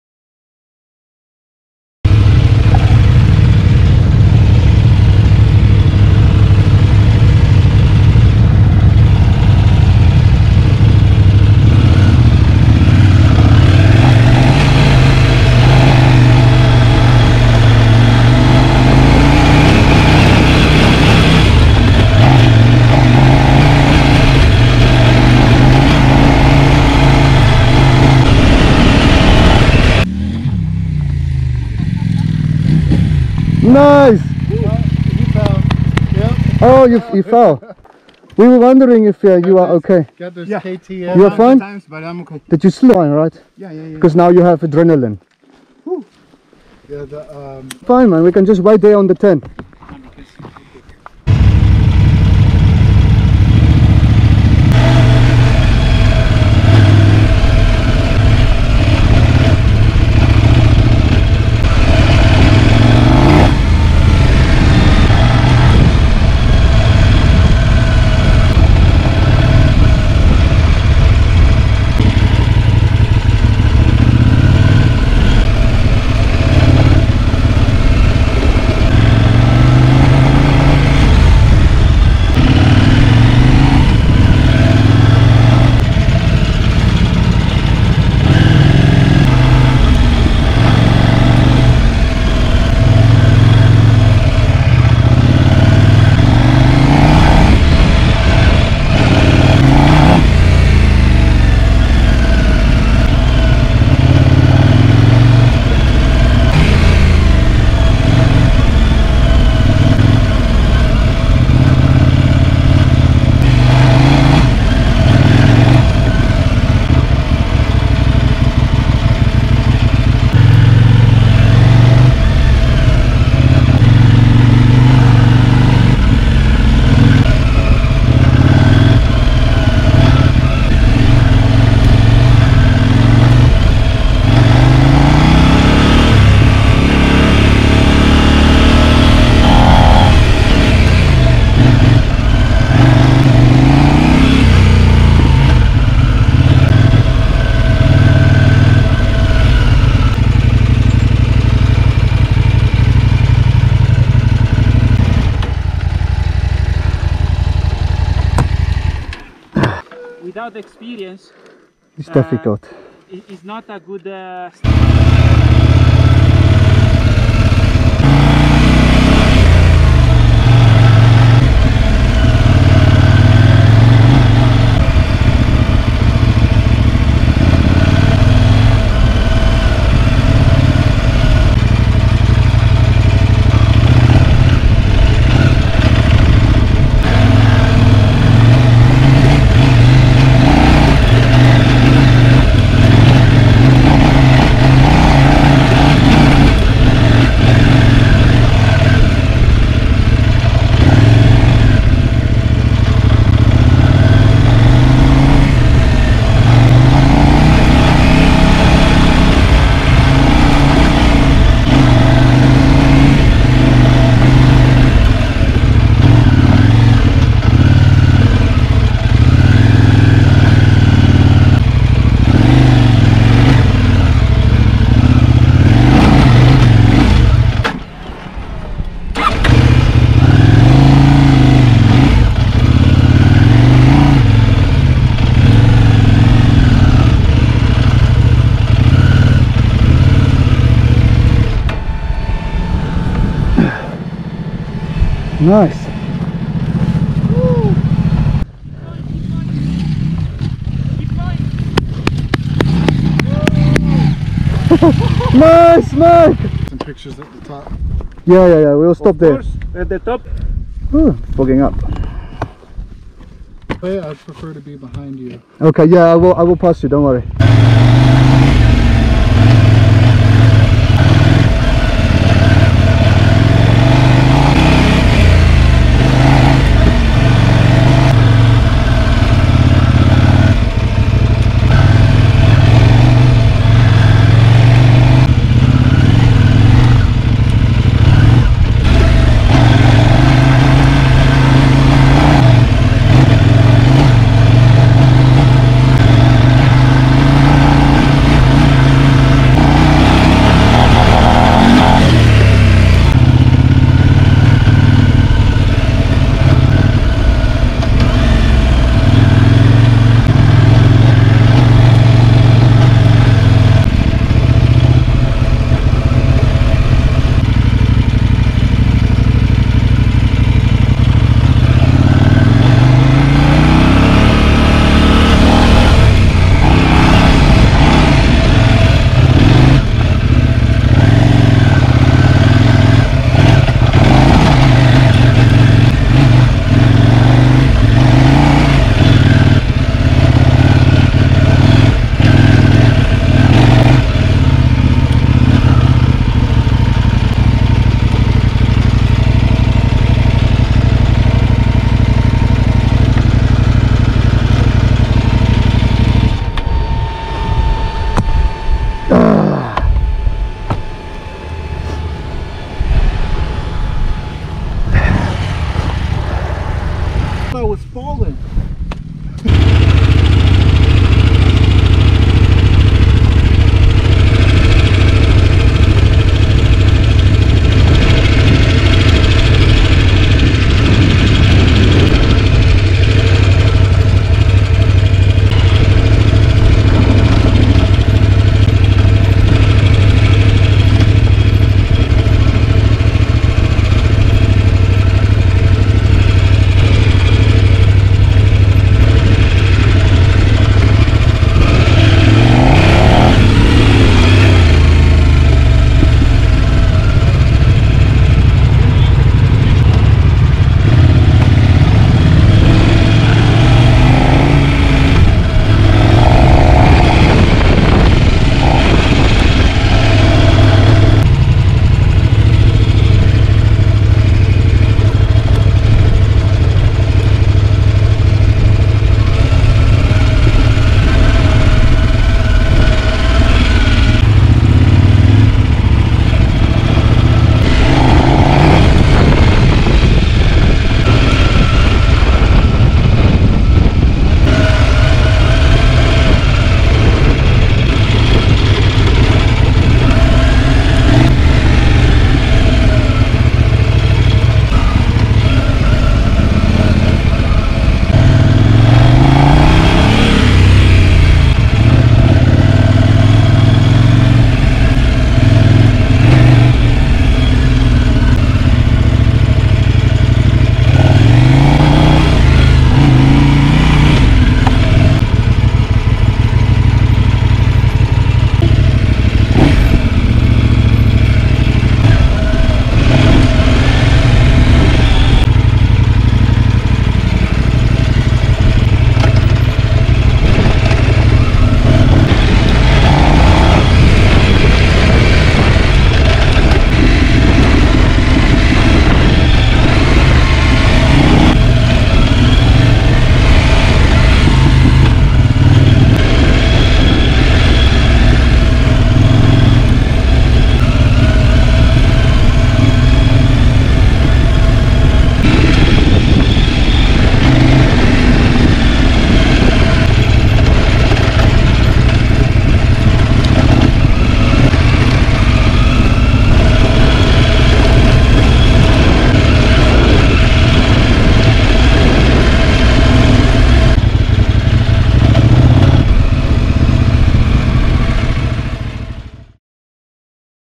Kove 800x, KTM790, Multistrada 950, CFmoto 800 off roading
The short version of our trip to the big rock, first time off roading for our friend on the MT800, and first time for the Multistrada to come here as well. Always great fun going up this road.